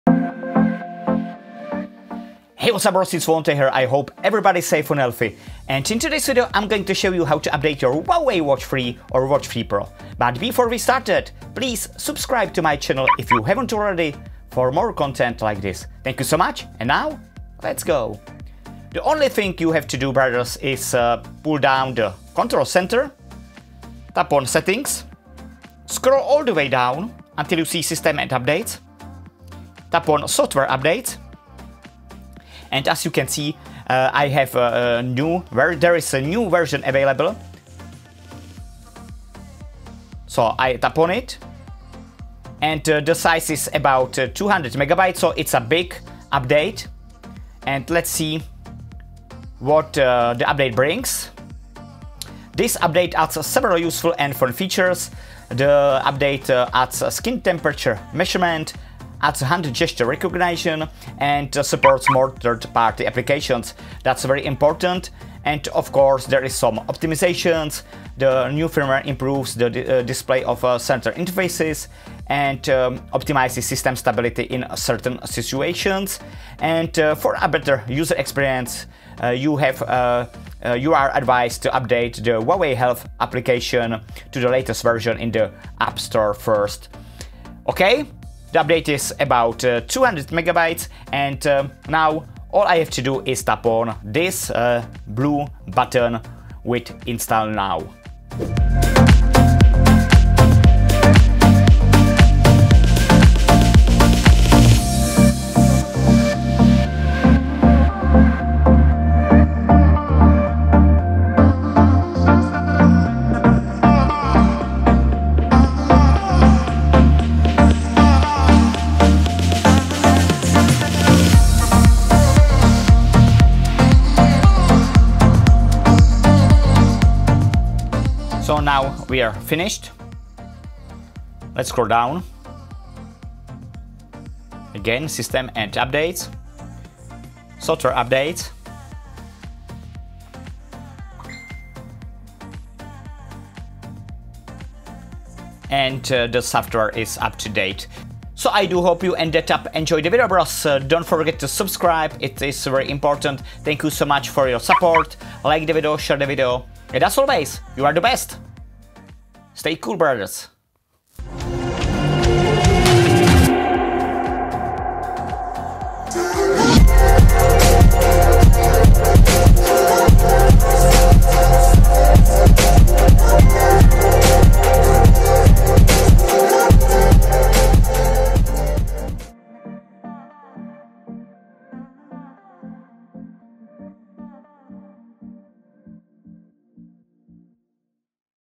Huawei Watch 3: How to Update Harmony OS! ⌚
A quick tutorial regarding how to update the software and OS of Huawei Watch 3 and Huawei Watch 3 Pro.